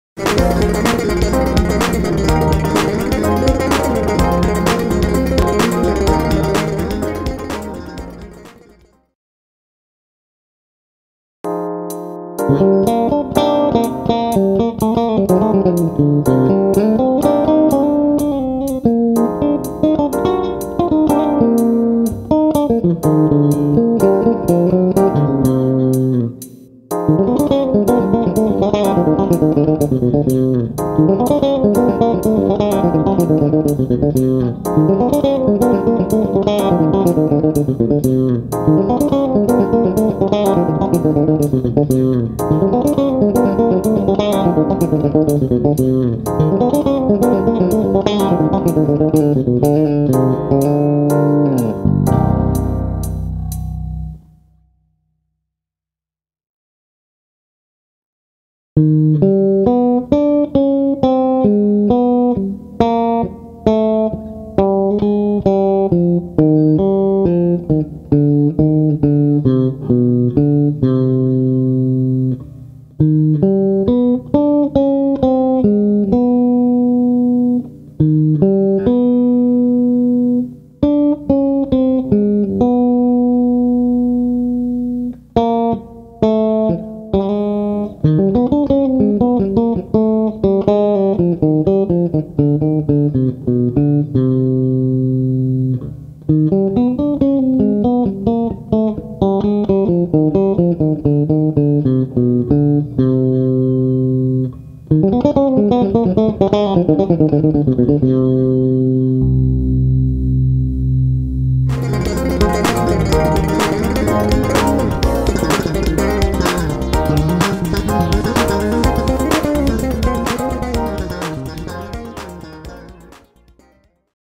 AMAZING JAZZ LICKS FOR BASS // Bebop Lesson
Enjoy these jazz licks for bass over Cm7
Learn the bebop jazz sound in this quick bass lesson.

Jazz licks bass lesson by  Fernando Petry
baixo z